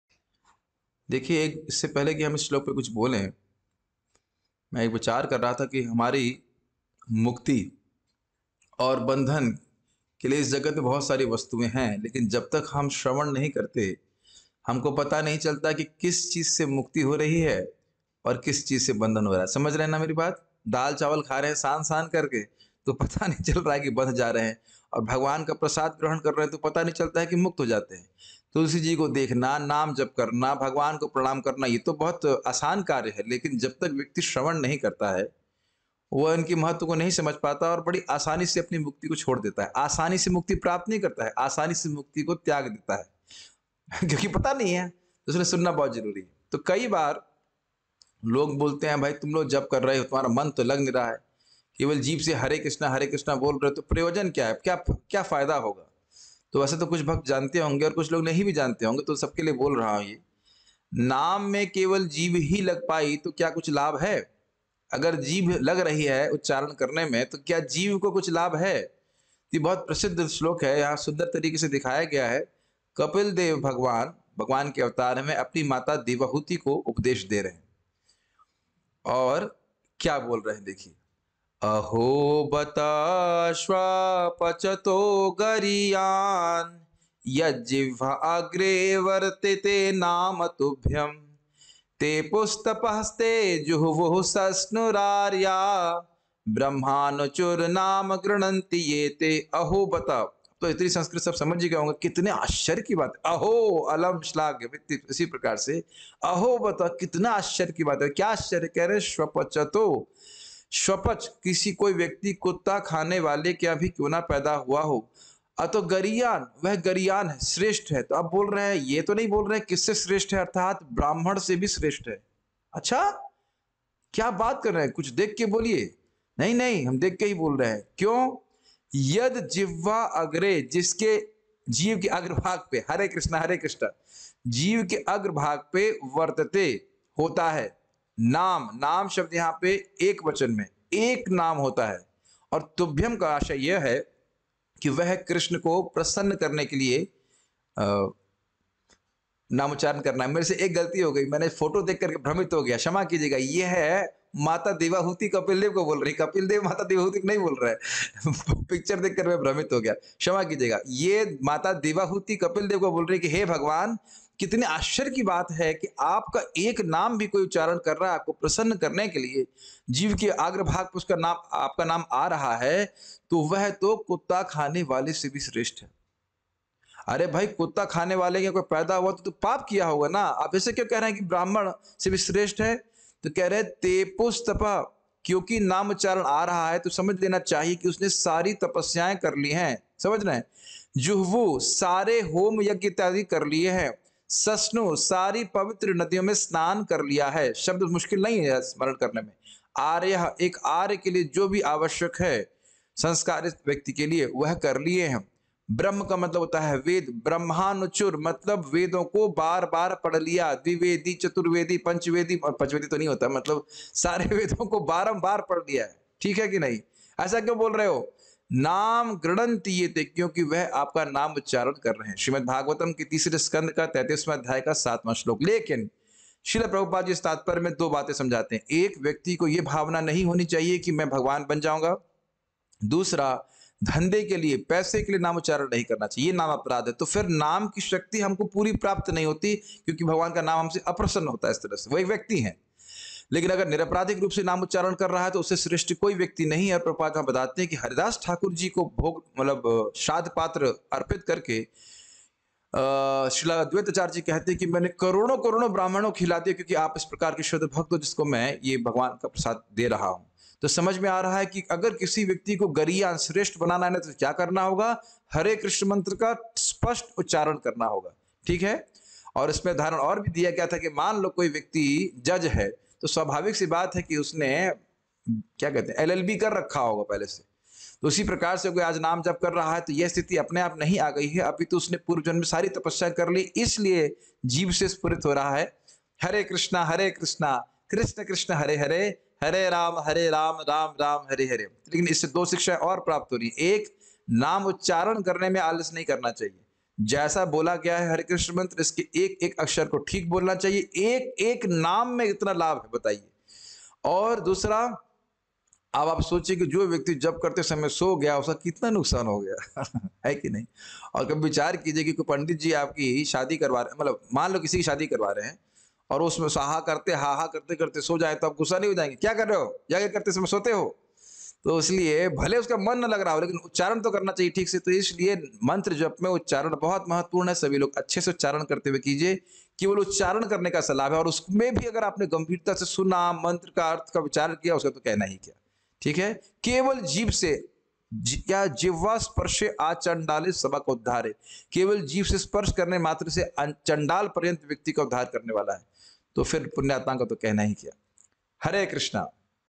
Japa talk 14 hindi | जप में मन नहीं लगता - क्या करें?
केवल जीभ से नाम बोलने का क्या लाभ जब मन लग ही नहीं रहा है ?